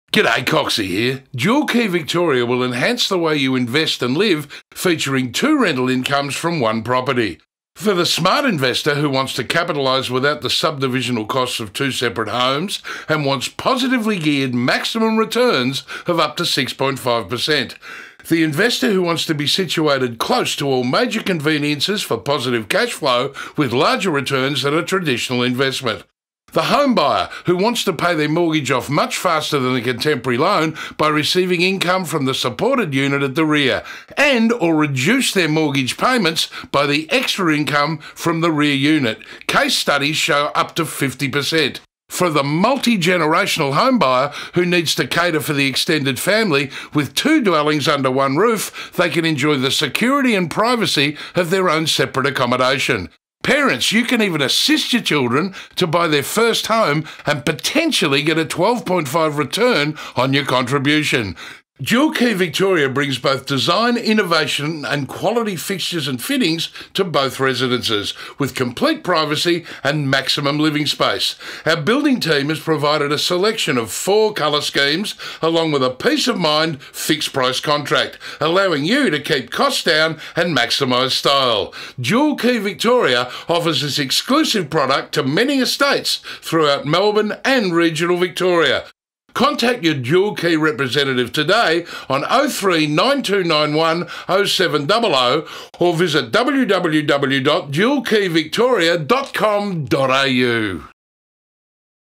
Dual Key Home - Walk Through
Dual Key Victoria presents our walk through video. Showcasing the front and rear unit. One Property. Two Incomes!